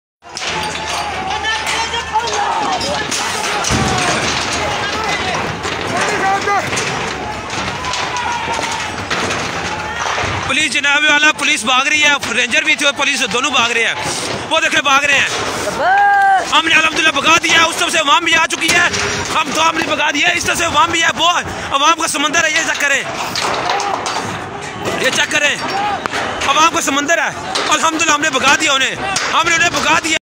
TLP supporters ny police ko bhaga diya,Rana Town Lahore.
Armed security forces chased away by tlp supporters-How could this be happening?